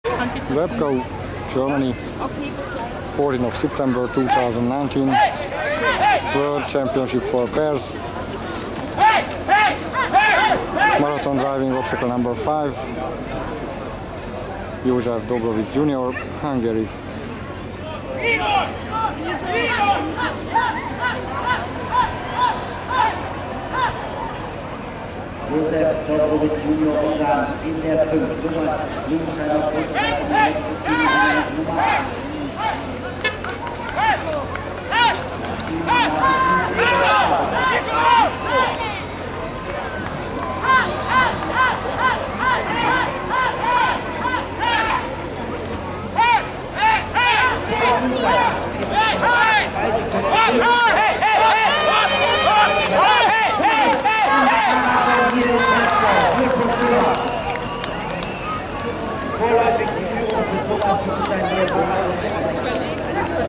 József DOBROVITZ Jr. (HUN) FEI CH-M-AH2, Drebkau (GER), marathon driving, obst. Nr. 5, 14.09.2019
József DOBROVITZ Jr. (Hungary)
FEI World Driving Championship for Pairs (CH-M-AH2), Drebkau (Germany), marathon driving, obstacle Nr. 5, 14 September 2019
ifj. DOBROVITZ József (Magyarország), FEI kettesfogathajtó világbajnokság, Drebkau (Németország), maratonhajtás, 5. akadály, 2019. szeptember 14.
lovak/horses: 42A: Indiaan - 42C: Tulip
segédhajtó/groom: CHARDON Bram